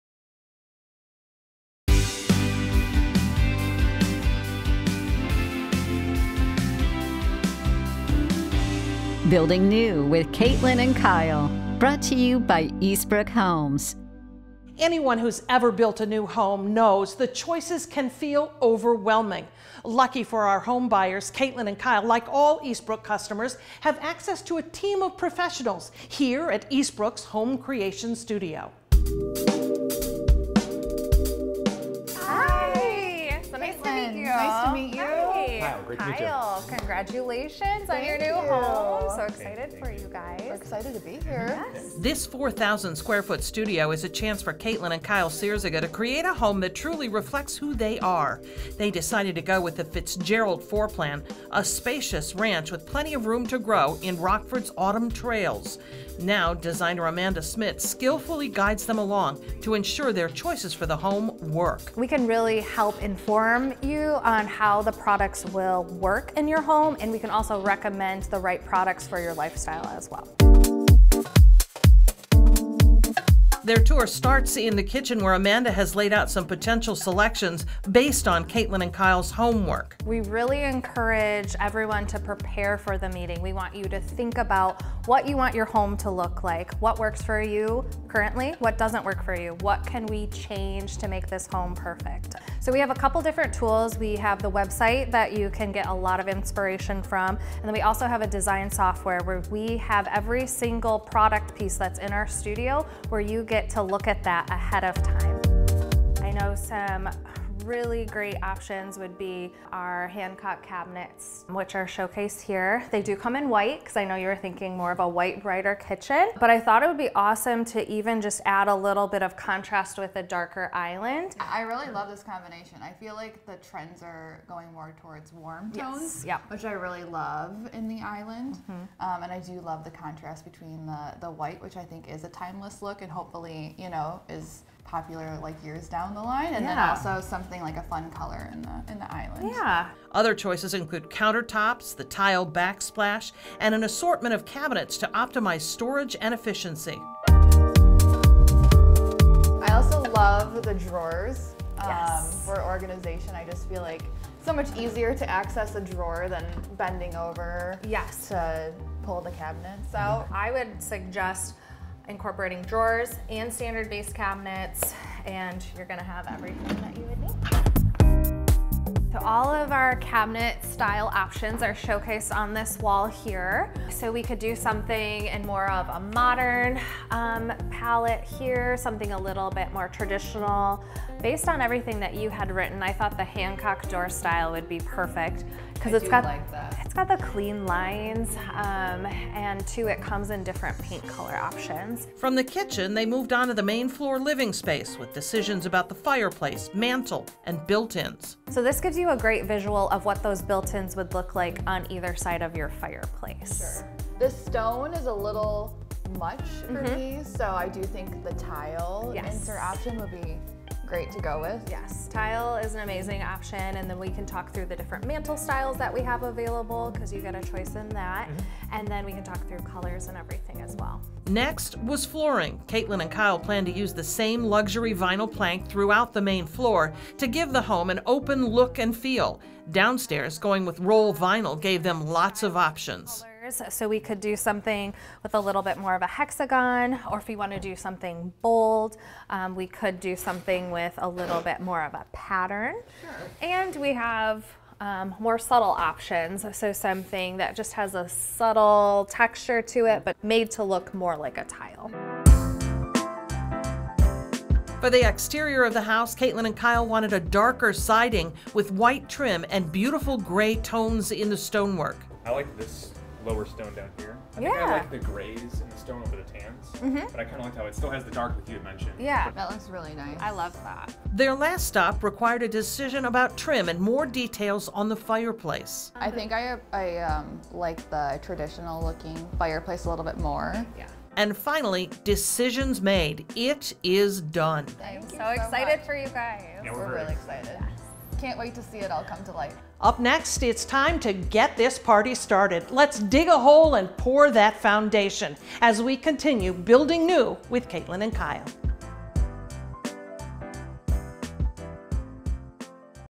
Sponsored: A visit to Eastbrook's Home Creation Studio provides expert advice on building new
Anyone who’s ever built a new home knows the choices can feel overwhelming.